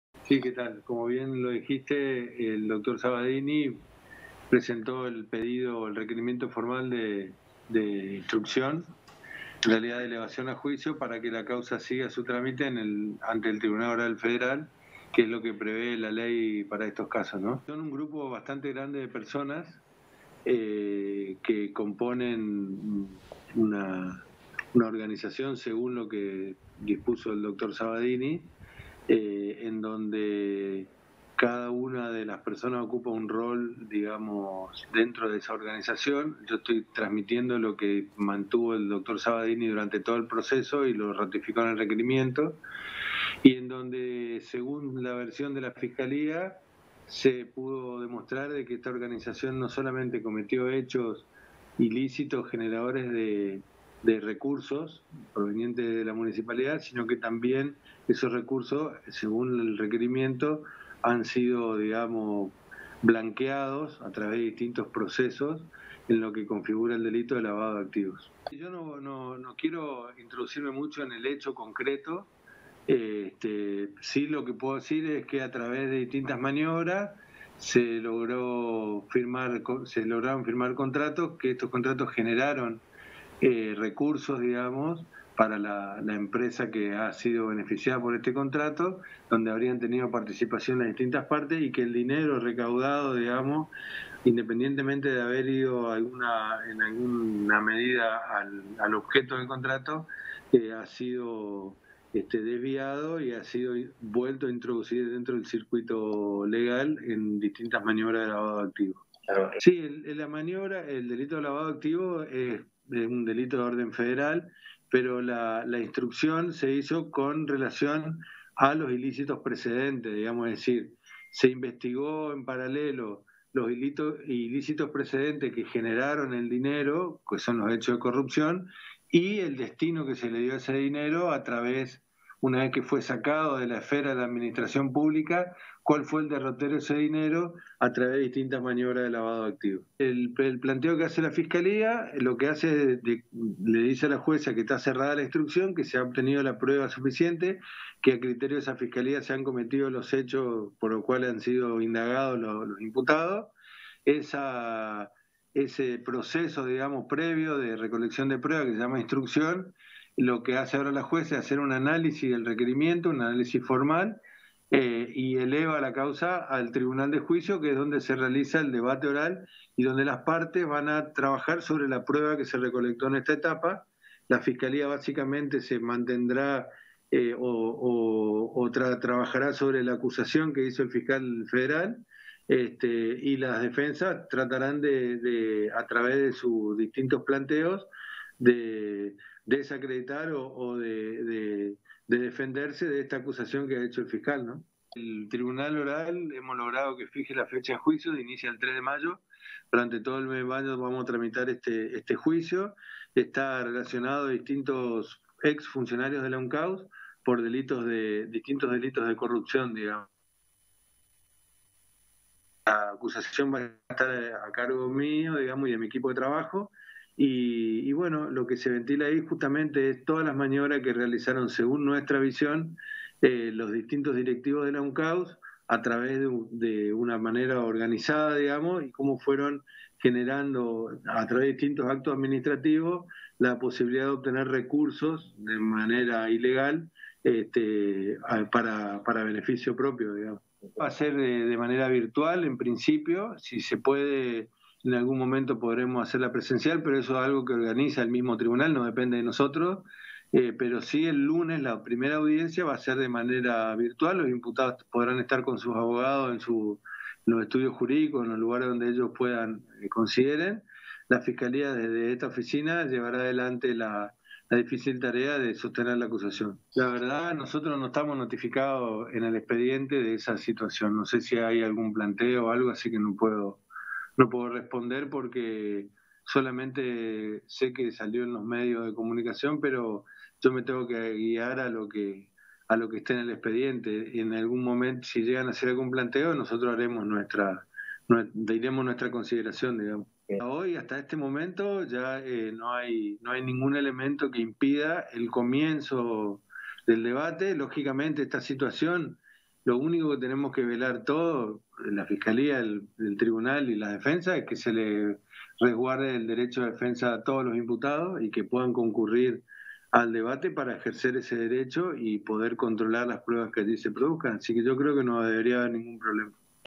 PEDIDO DE ELEVACIÓN DE JUICIO LAVADO II
FEDERICO CARNIEL - FISCAL FEDERAL
El Fiscal Carniel se refirió al pedido del Fiscal Patricio Sabadino a la Jueza Zunilda Niremperger para elevar a juicio la causa que los tiene como principales imputados a Aida Ayala y a Jacinto Sampayo.